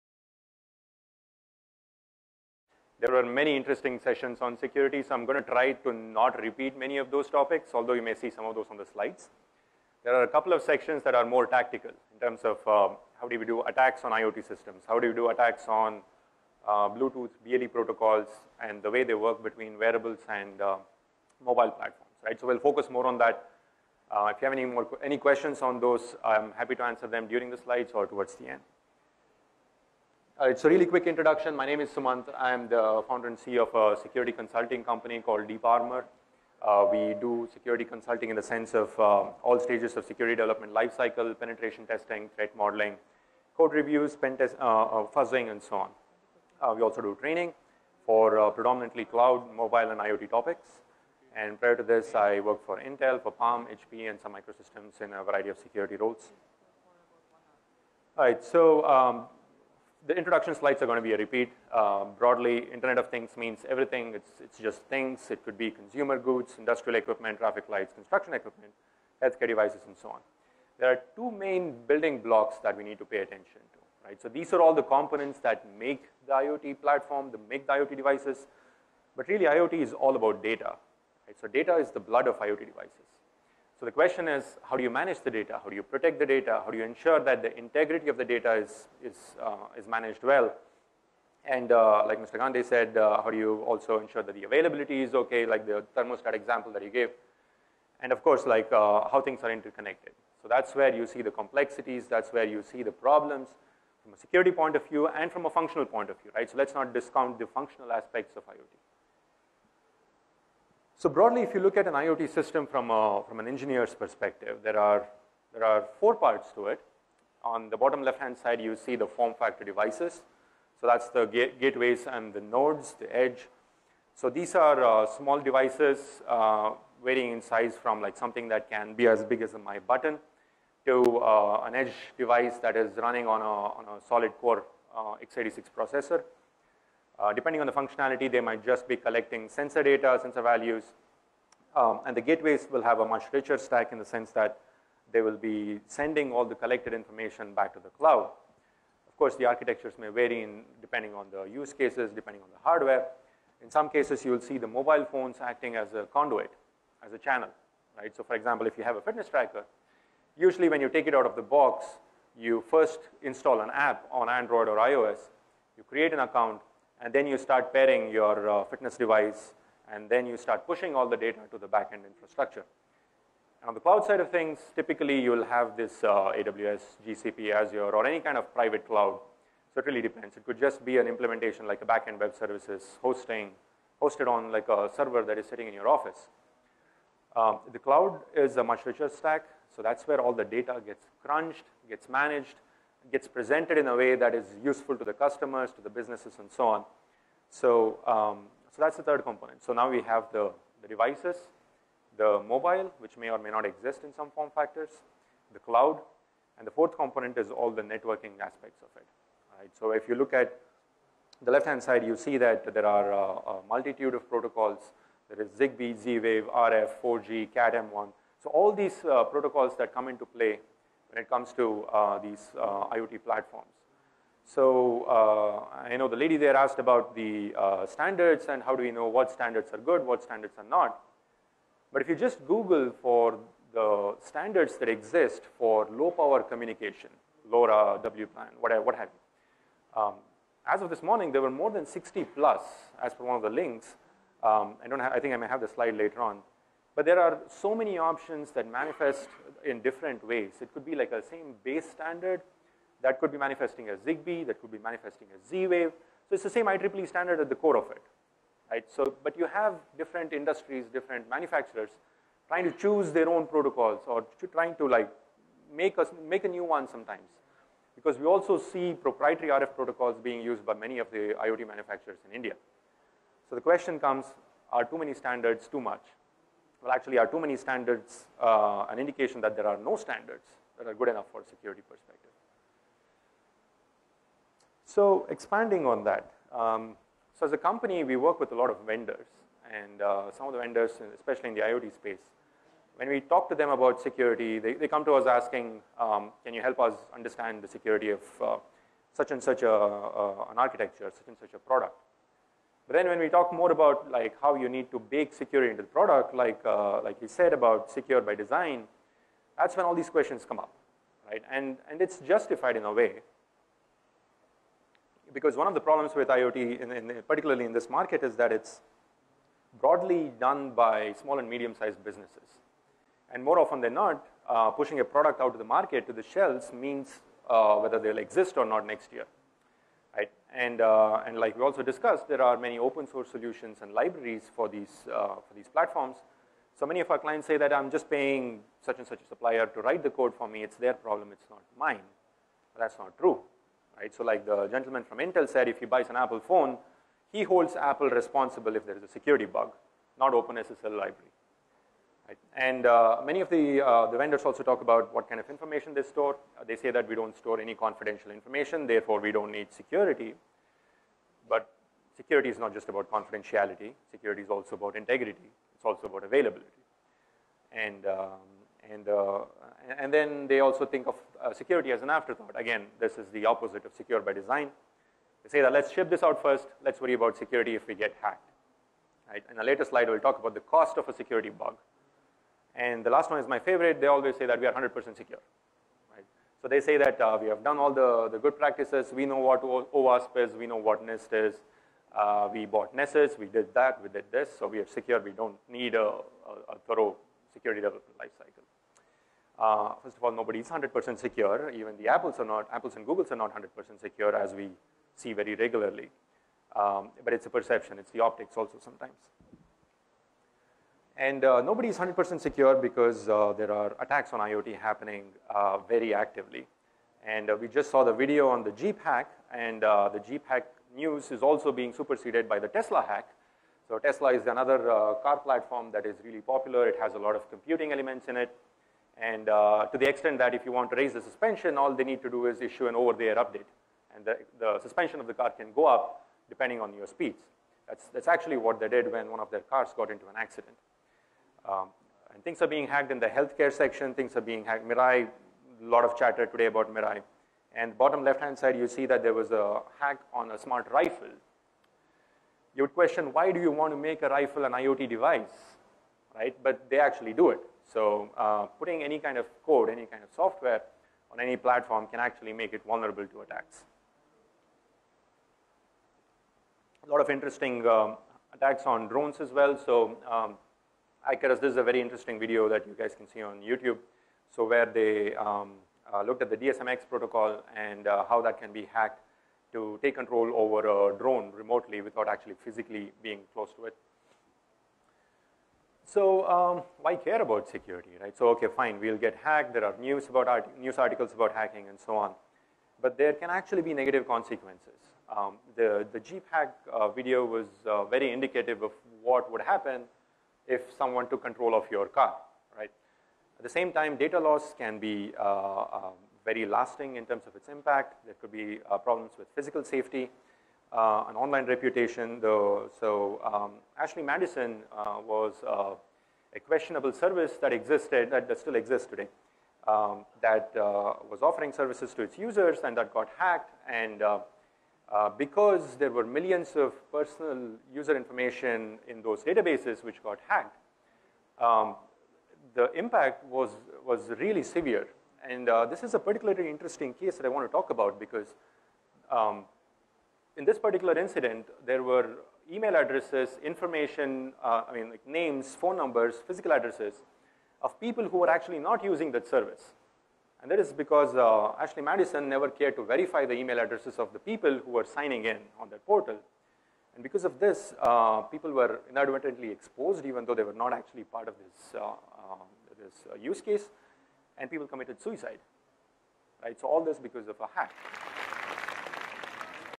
PERILS OF TRIVIALIZING IoT SECUIRTY: WHY SHOULD YOU CARE AND WHAT CAN YOU DO?
The Internet of Things (IoT) market today is defined by product manufacturers pushing a broad spectrum of computing devices out to the hands of consumers at an ever-increasing pace.

Watch the talk by Sumanth Naropanth, CEO, Deep Armor to learn the steps that IoT vendors can take to mitigate common security problems.